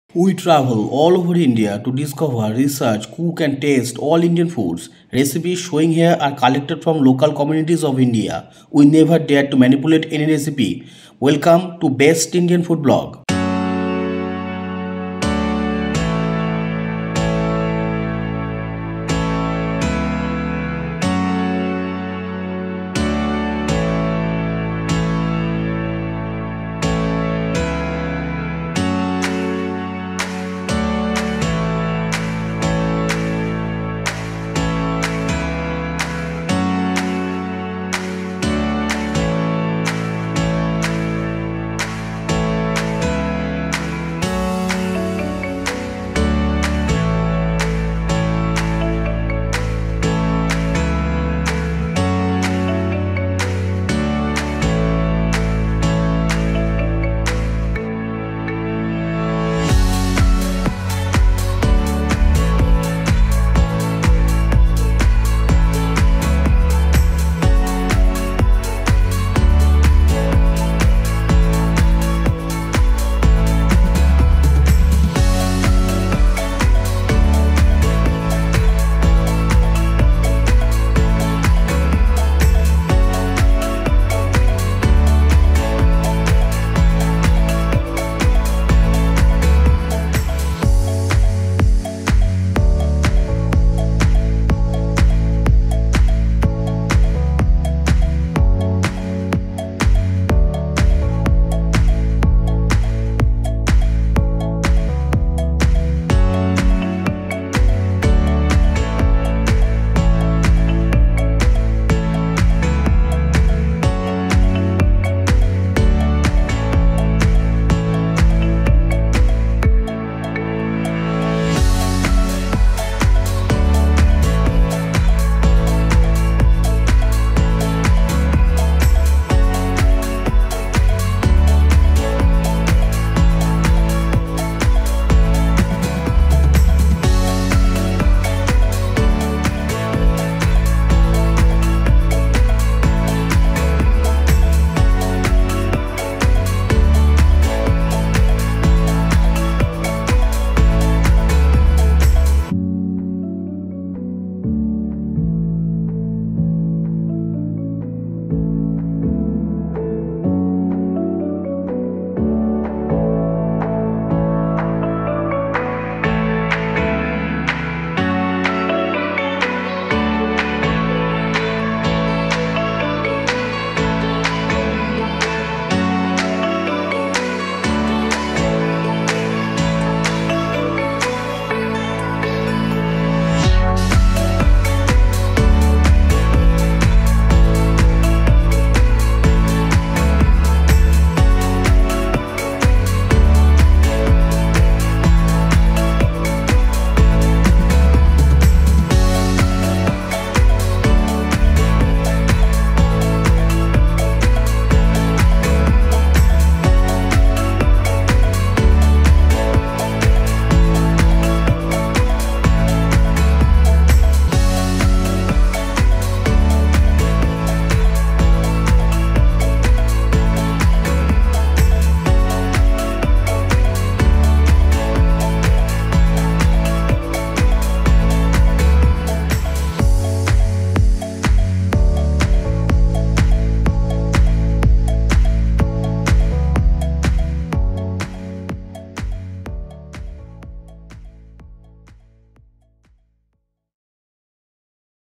Galho from Nagaland, India | Best Indian Food Blog | Rice Vegetable recipe
Ingredients : Rice, Asparagus Bean, Cabbage, Mustard Leaves, Tomato, Ginger, Garlic, Onion, Green Chili, Salt, Water.

Recipe at a Glance : Chop asparagus bean, tomato, ginger, garlic, green chili, onion ⇒ boil water ⇒ put rice, chopped asparagus bean, cabbage, mustard leaves, onion, garlic, ginger, green chili ⇒ boil ⇒ add chopped tomato, salt ⇒ boil with lid to tender.